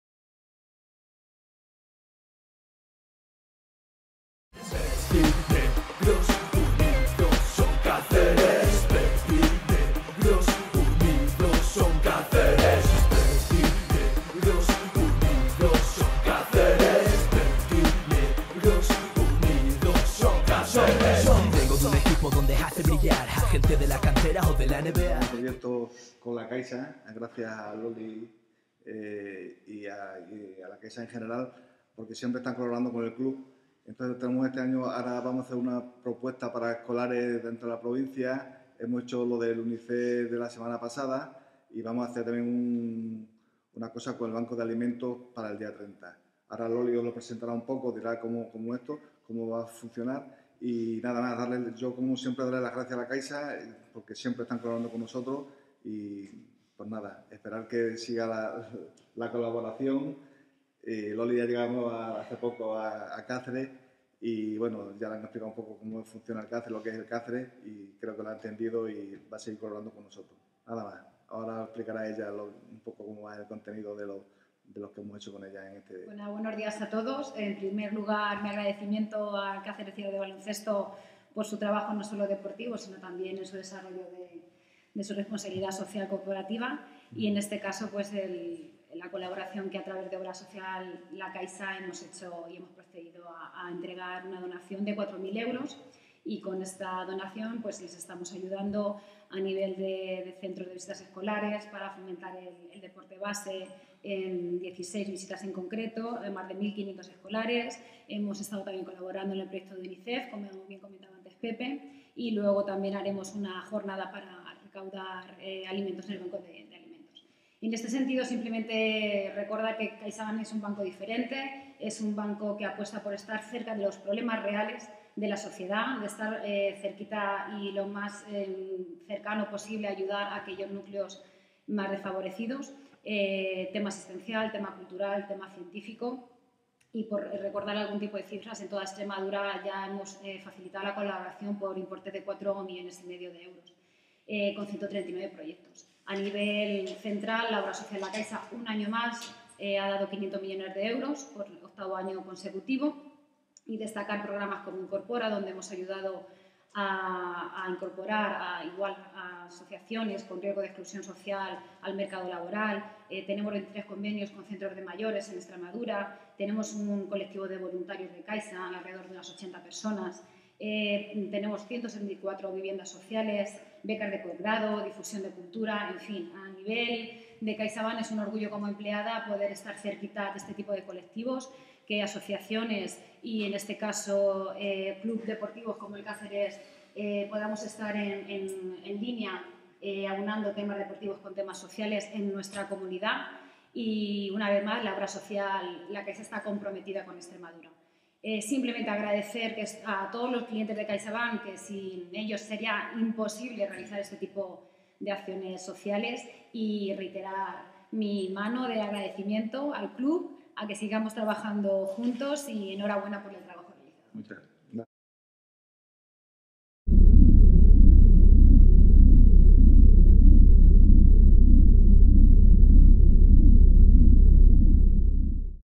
t1617pj16 acuerdo caixa
Caceresbasket 2016/17 El presidente del Cub, José Manuel Sánchez, y la directora provincial de Banca de Empresas de Caixabank, Loli Garrido, comparecen ante los medios de comunicación para presentar algunas de las acciones del DiciembreSolidario dentro de la colaboración del CLub y la Obra Social la Ciaxa